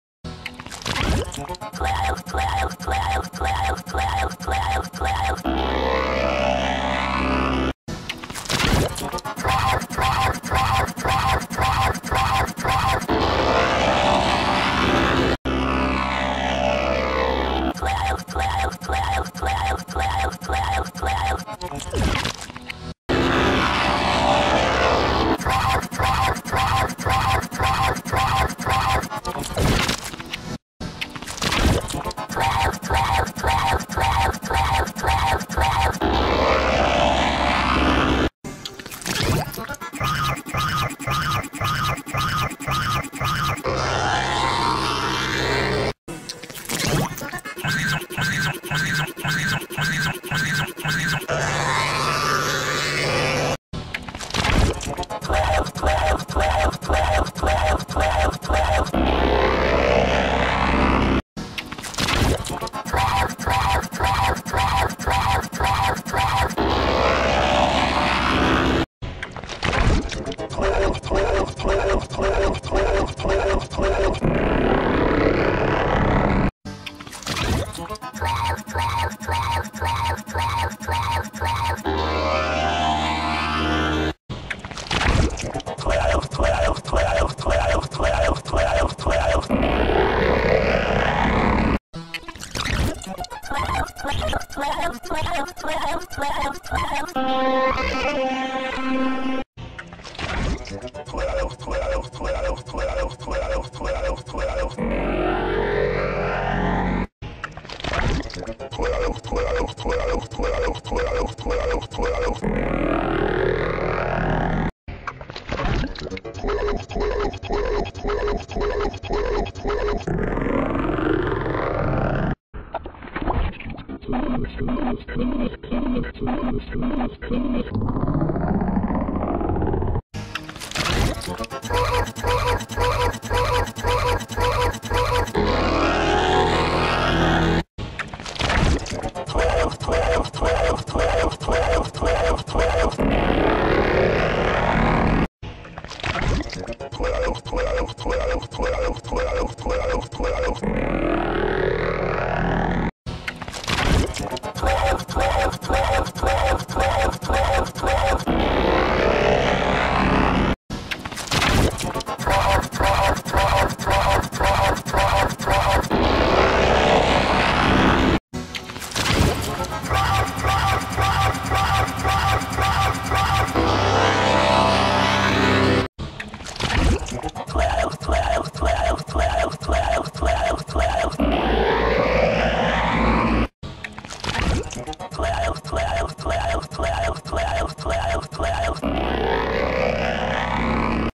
Wall Of Klaalk Effects (Inspired By Preview 2 Loud Walker Effects)
Edit: When Vegas crashed while editing, the effect chain for Zoopals Effect V3 was not saved after applying Spherize and closing it, and it contained Pinch/Punch after I restored it, and I did not notice it until now…